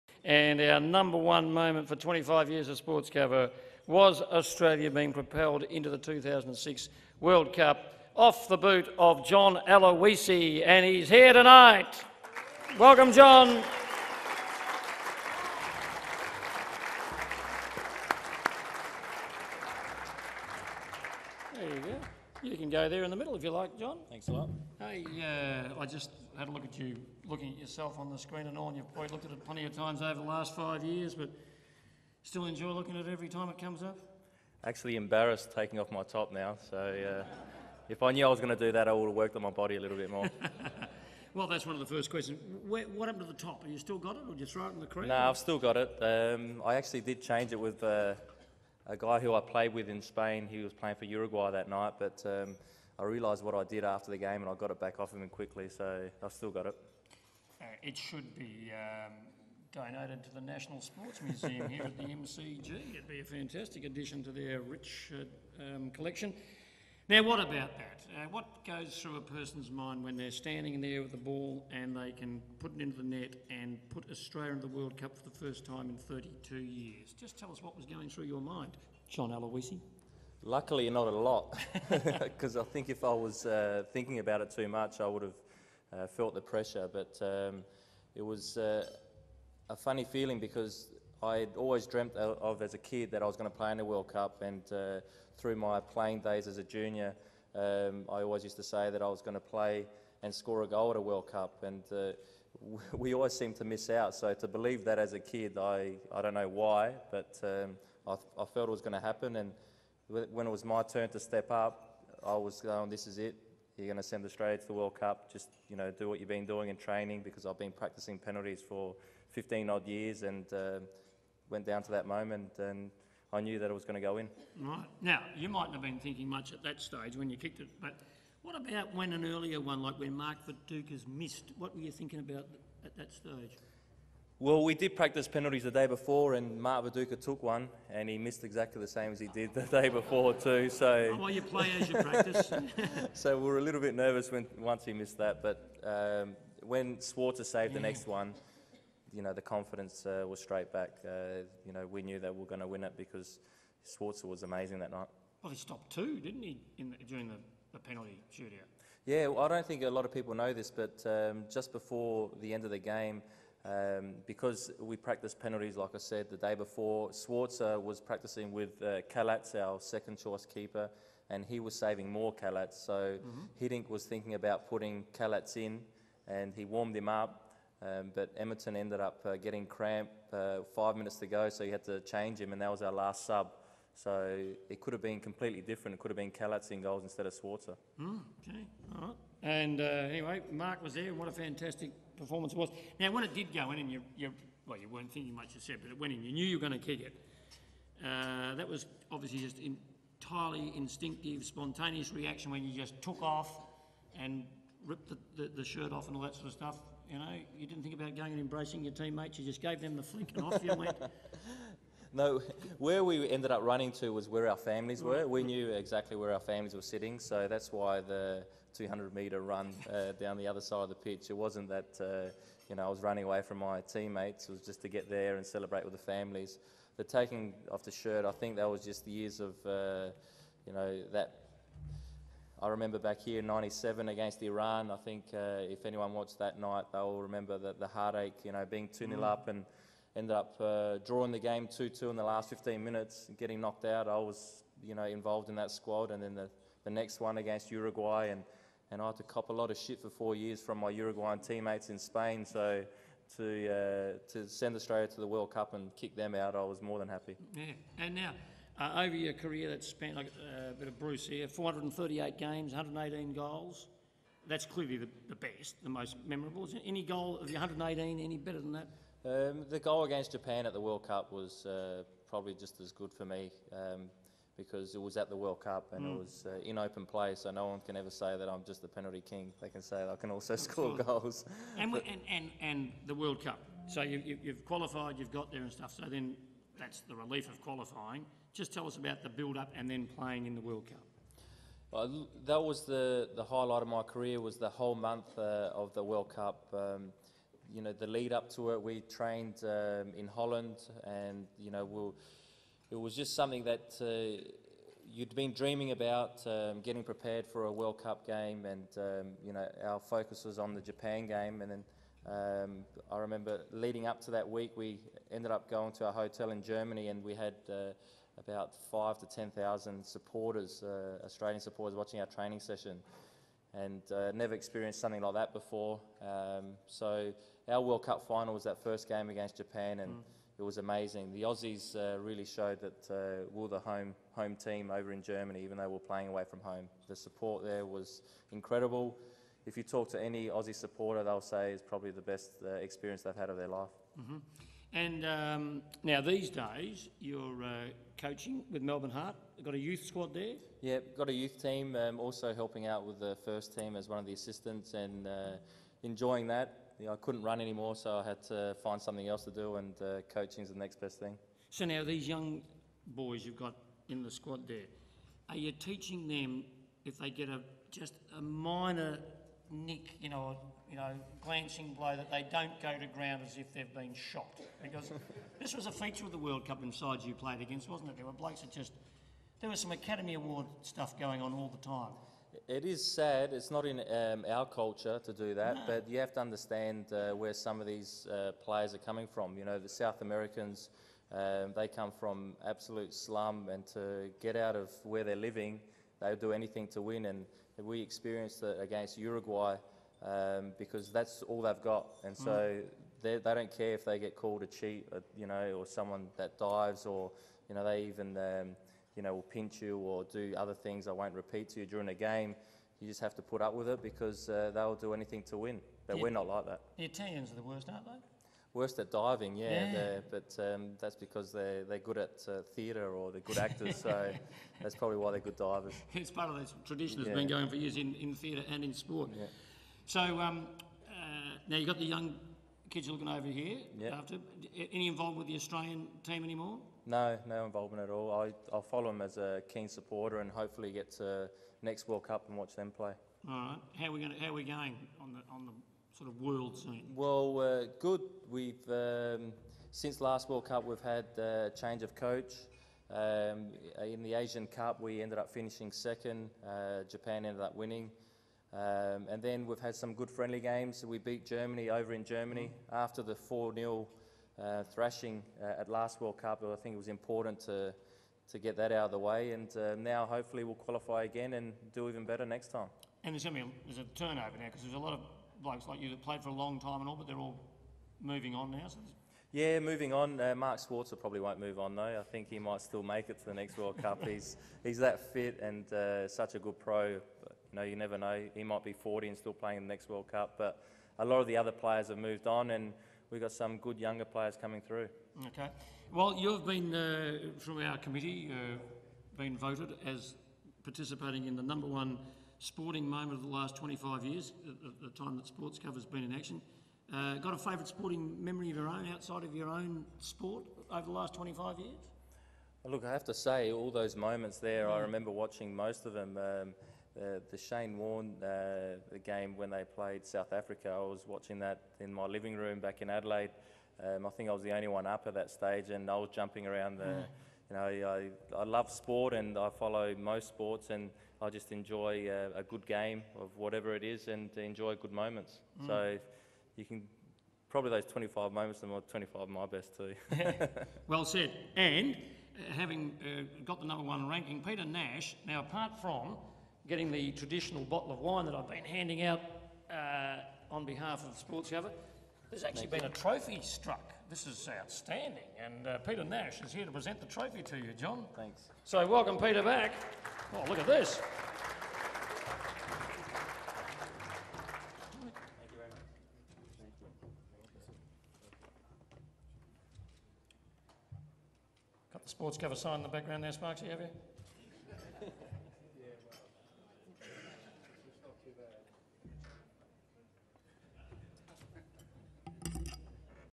Sportscover's 25th Year Anniversary Gala Dinner - with John Aloisi
John Aloisi speaking at Sportscover's 25th Year Anniversary Gala Dinner about scoring the goal that took Australia to the 2006 World Cup